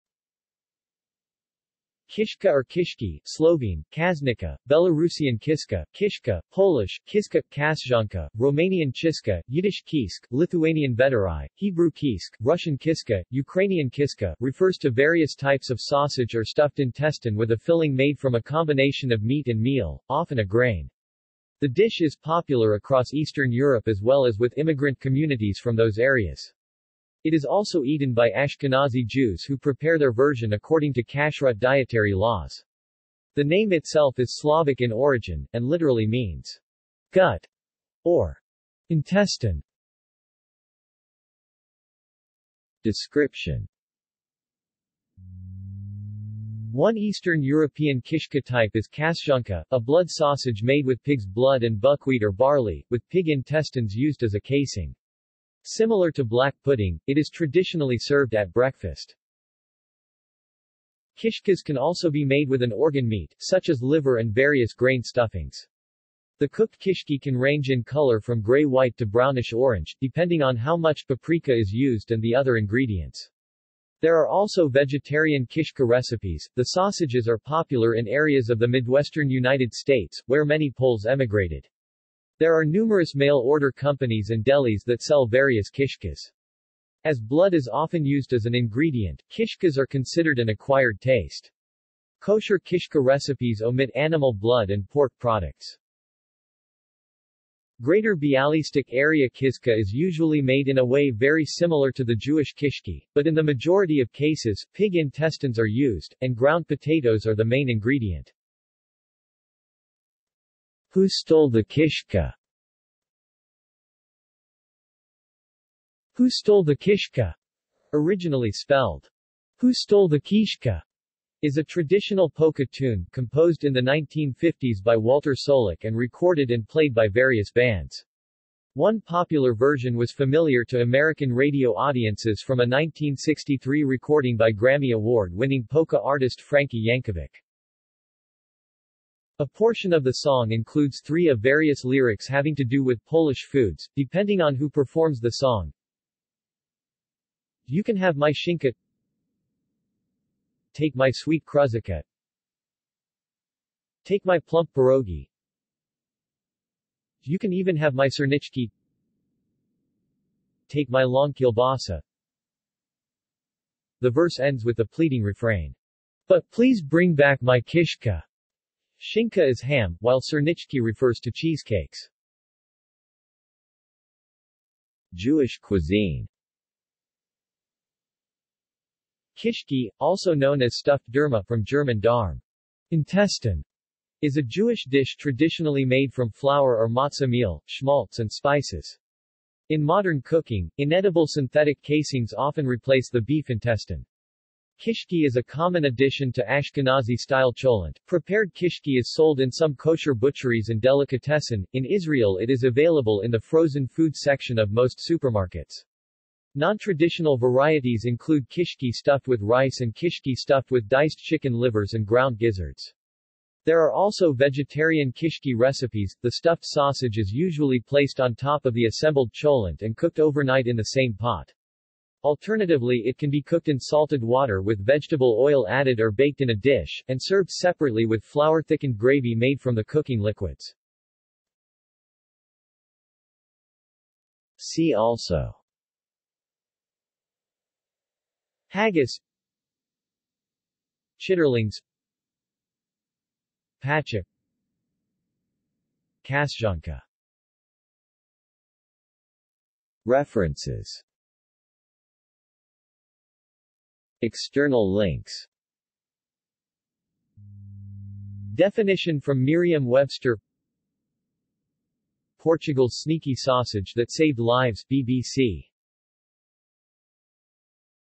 Kishka (food) | Wikipedia audio article
This is an audio version of the Wikipedia Article:
Kishka (food)

SUMMARY
Kishka or kishke (Slovene: kašnica; Belarusian кішка, kishka; Polish: kiszka / kaszanka; Romanian chişcă; Yiddish קישקע; Lithuanian vėdarai; Hebrew קישקע; Russian кишка; Ukrainian кишка) refers to various types of sausage or stuffed intestine with a filling made from a combination of meat and meal, often a grain. The dish is popular across Eastern Europe as well as with immigrant communities from those areas. It is also eaten by Ashkenazi Jews who prepare their version according to kashrut dietary laws. The name itself is Slavic in origin, and literally means "gut" or "intestine."